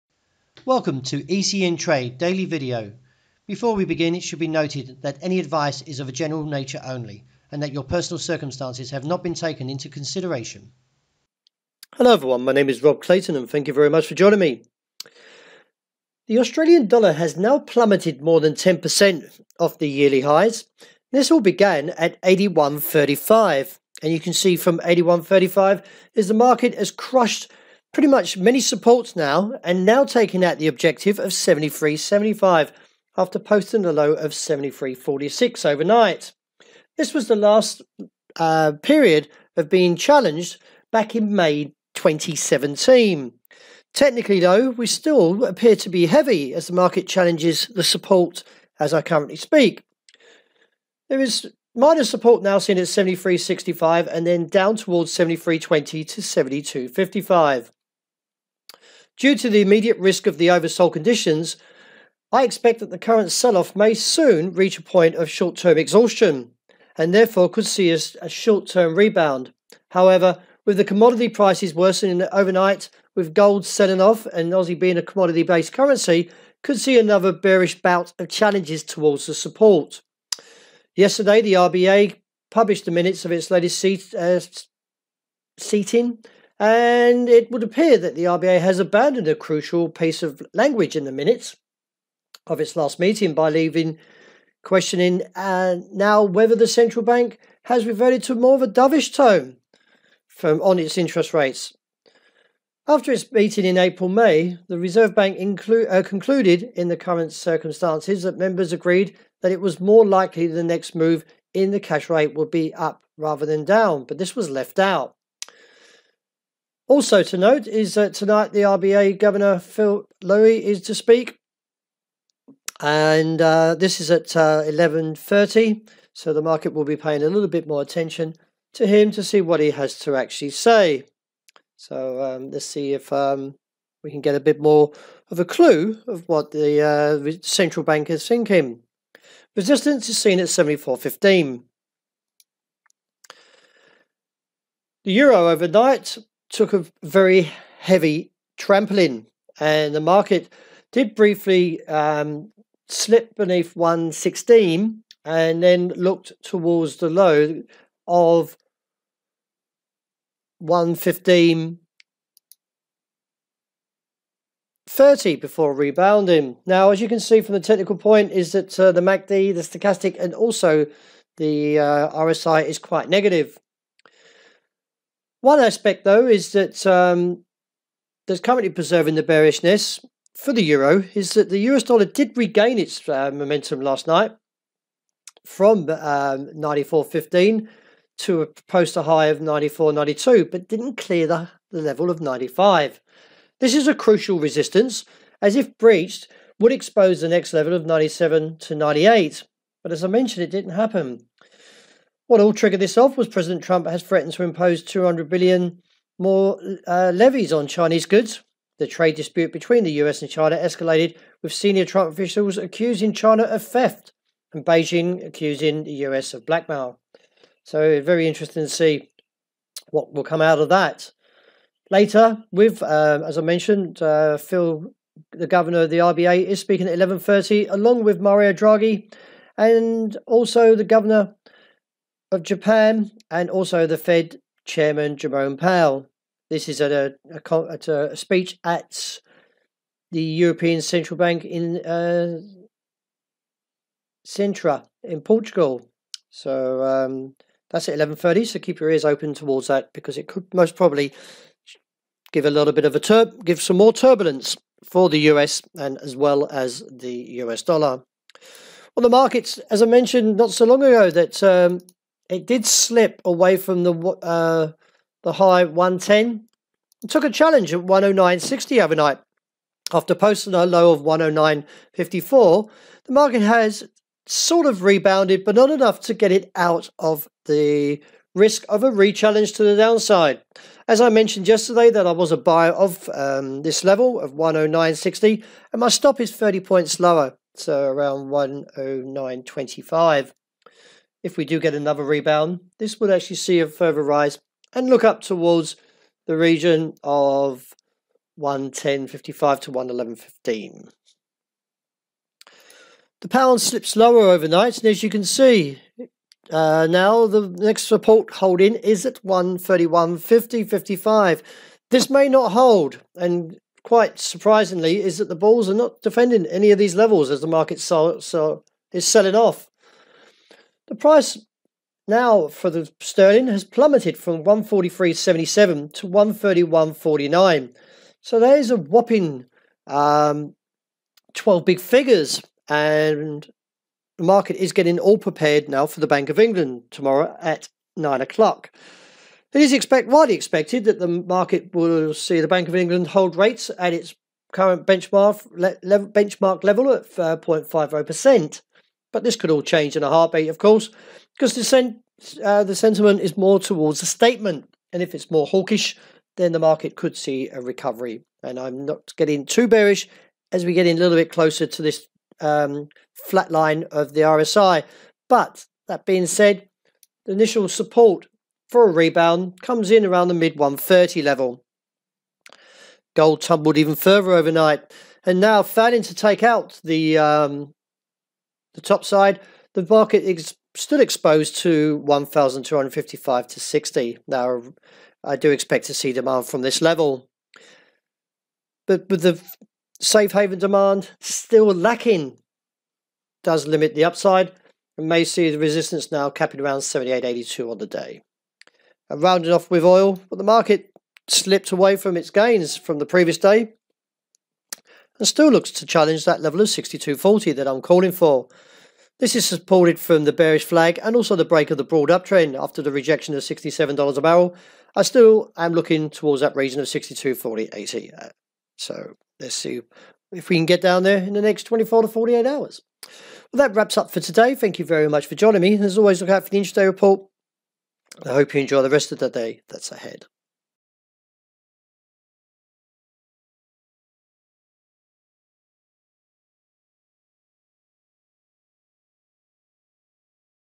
ECN Trade Technical Webinar 20 Jun 2018
The Australian Dollar has now plummeted more than -10% off the yearly highs (0.8135) after the overnight’s decline crushes the objective of 0.7375 and posted a low of 0.7346 (last witnessed early May 2017) and may now be approaching initial support targets of interest from either 0.7320, to 0.7255.